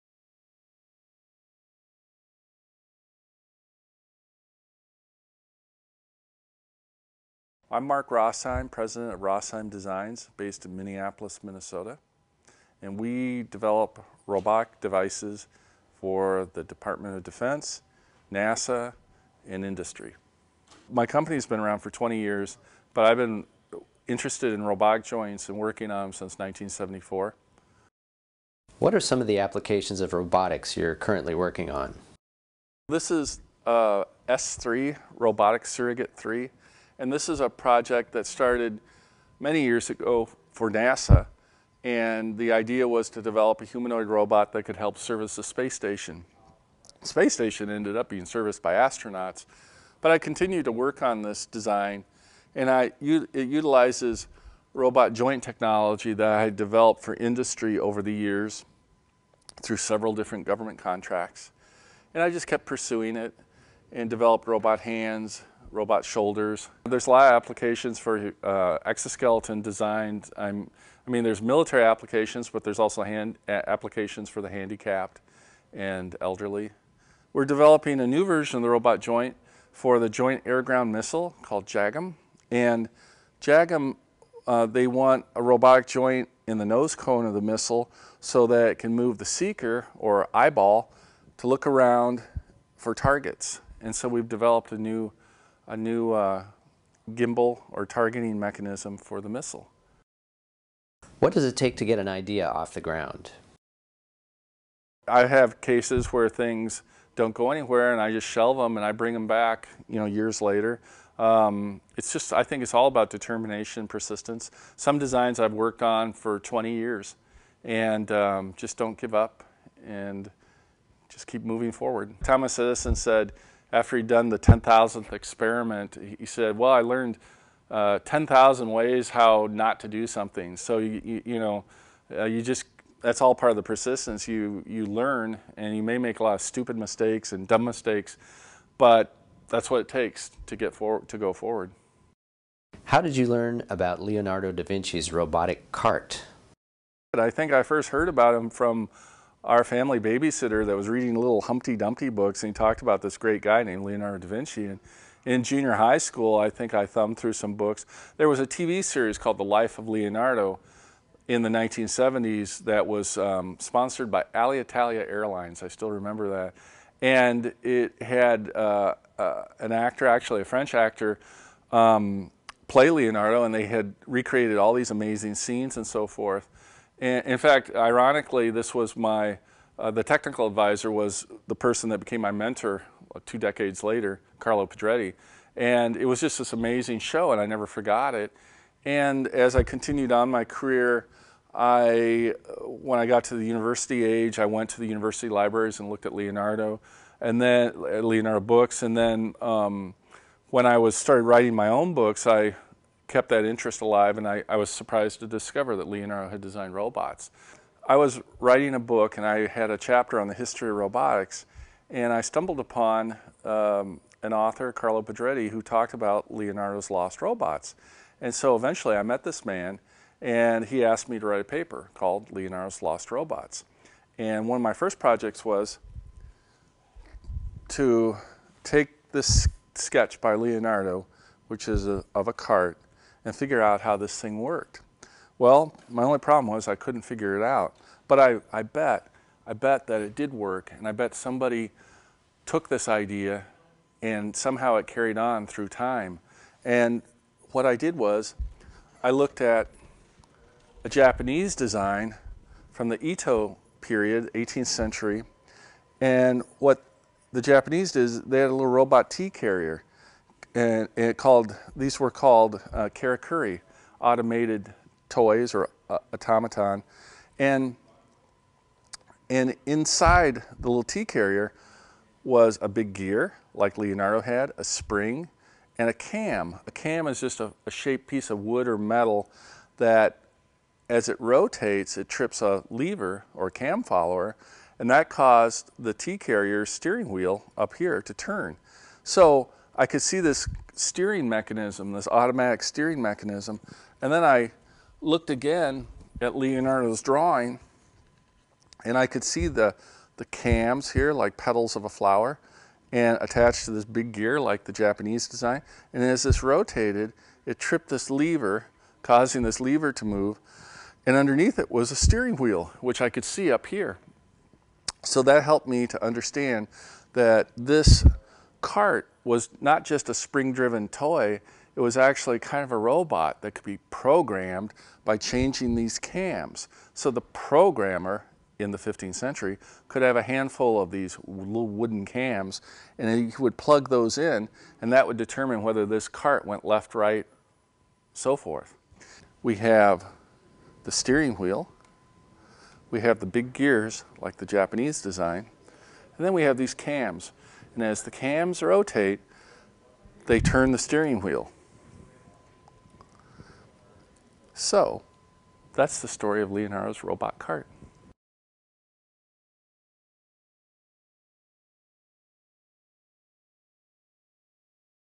Robots from Leonardo to the present: a video interview with Mark Rosheim
Mark Rosheim is founder and president of Ross-Hime Designs, Inc., a Minneapolis, Minnesota-based mechanical design company. He is the author of three books on robotics, most recently Leonardo's Lost Robots (Springer, 2006).

He was interviewed for SPIE Newsroom at the SPIE Defense and Security Symposium by Kevin Probasco and Beth Kelley.

Ross-Hime Designs has won seven Phase I Small Business Innovation Research (SBIR) and six Phase II SBIR contracts for mechanical motion technology invented by Mr. Rosheim. His wrist design, the "Omni-Wrist," has been commercialized for spray finishing and sealant application.

Rosheim attended the University of Minnesota, studying mechanical engineering. He has been employed by Honeywell and Unisys corporation research departments.